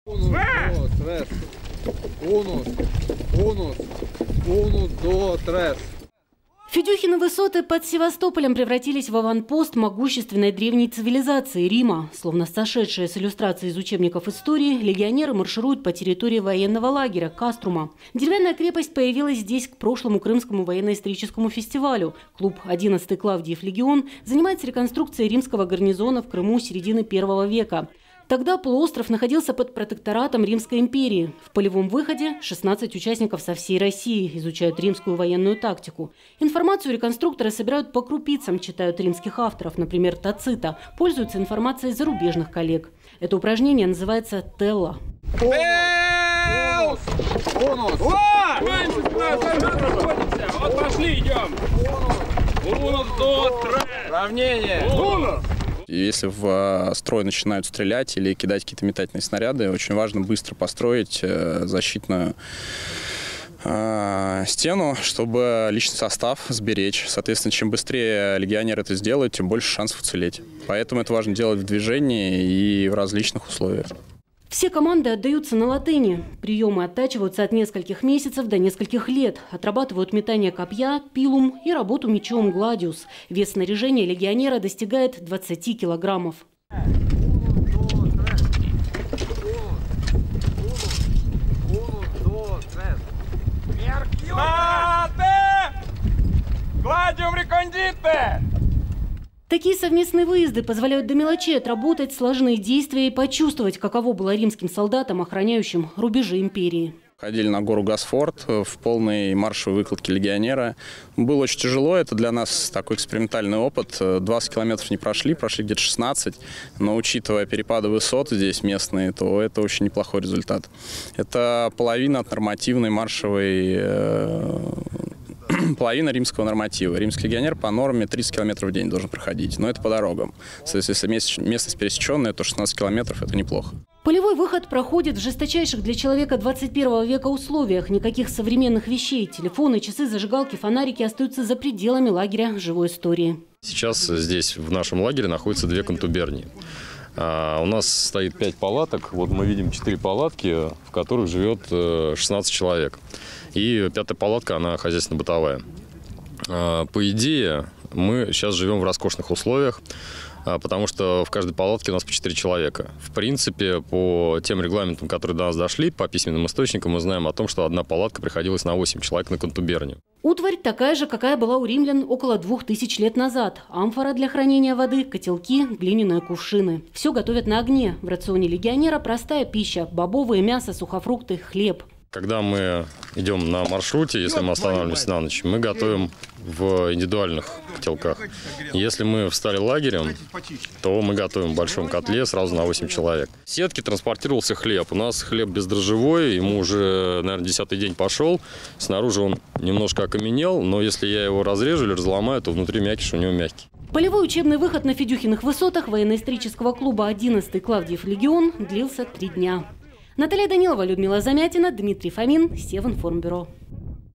16.05.2016 Полевой выход реконструкторов
На Федюхиных высотах состоялся полевой выход реконструкторов эпохи Древнего Рима. В течение четырех дней 16 легионеров полностью погрузились в прошлое.